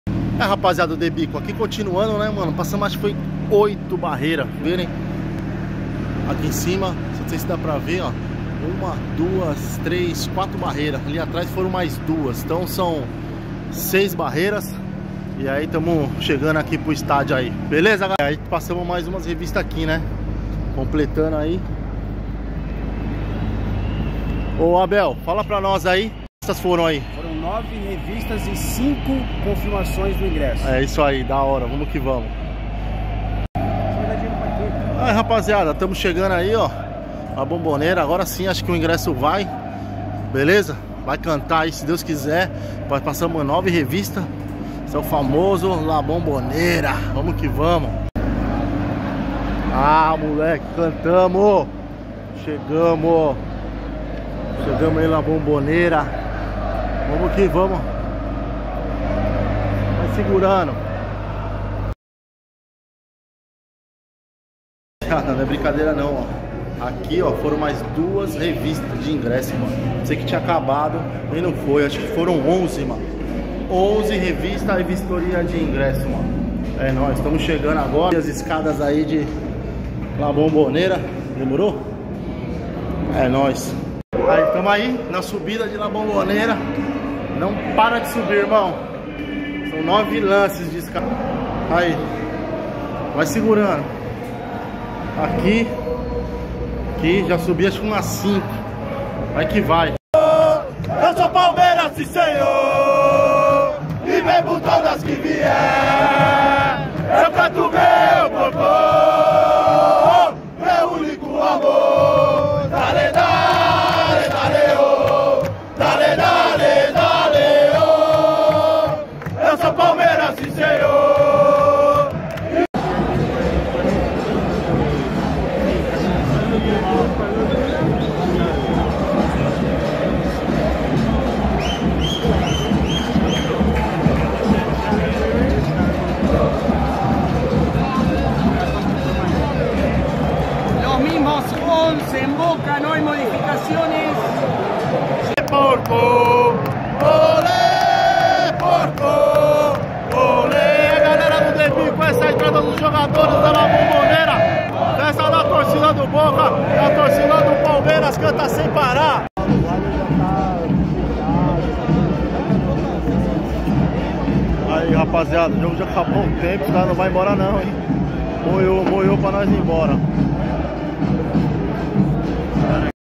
VEJA COMO FUNCIONA AS REVISTAS E ACESSO A LA BOMBONERA - BOCA JUNIORS x PALMEIRAS - BAITA ESCADARIA
De bico mostra o difícil acesso até as arquibancadas do lendário estádio La Bombonera do time Boca Juniors.